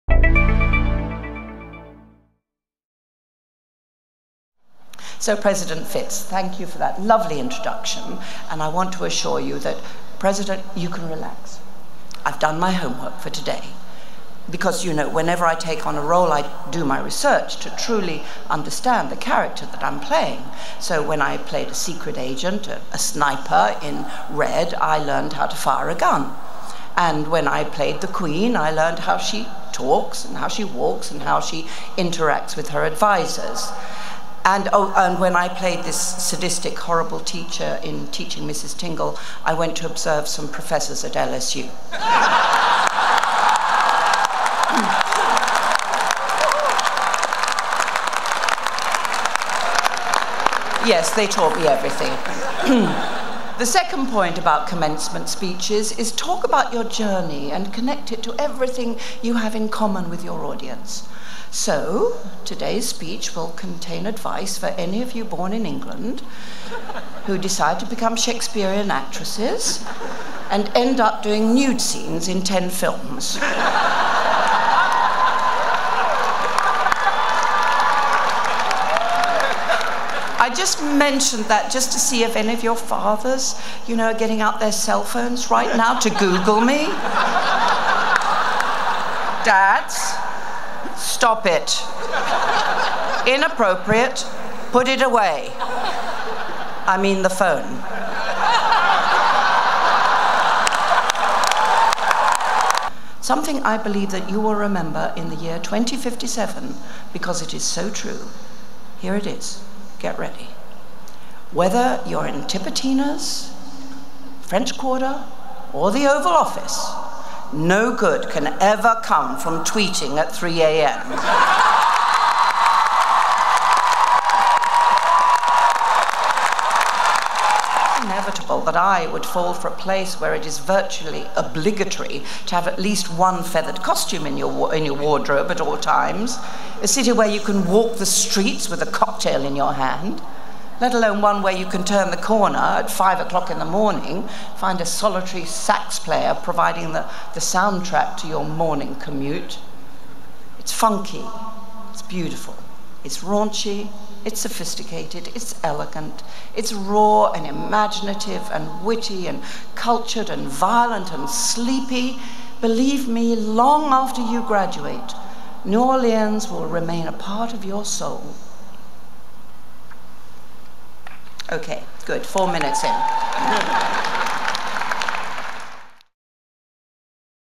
Helen Mirren's Tulane University commencement address
Actress Helen Mirren gives the commencement address during Tulane University's commencement ceremony at the Superdome on Saturday, May 20, 2017.